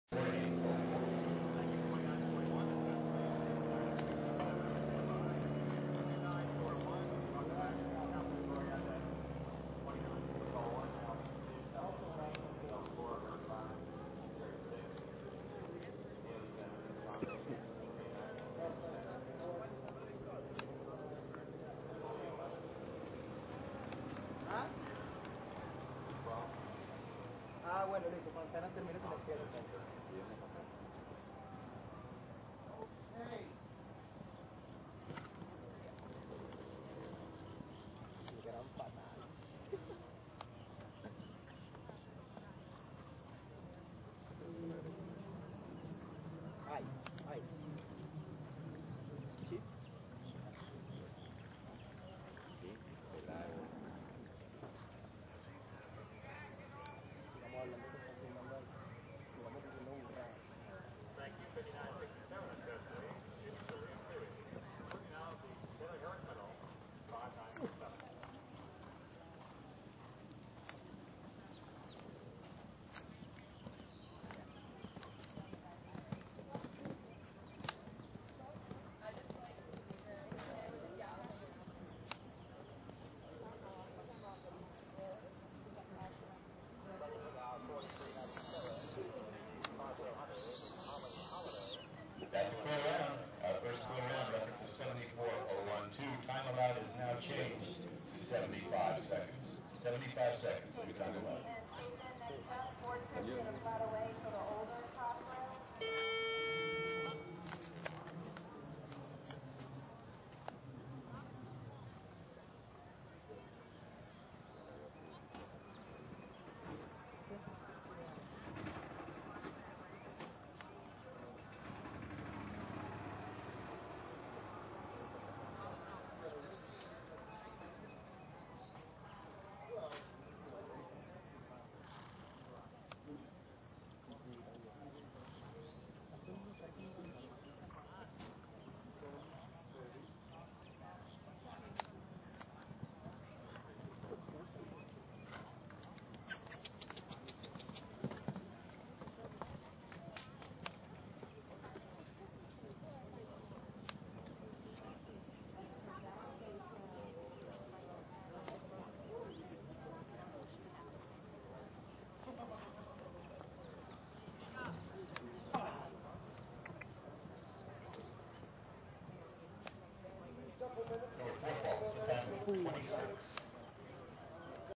Video of FATALIS FATUM ridden by STEVEN BLUMAN from ShowNet!
FATALIS FATUM ridden by STEVEN BLUMAN at WEF 10 CSI 2*.  This round is from class 1035, $10000 Charles Owen Low AO Jumper Classic and finished 12 of 51.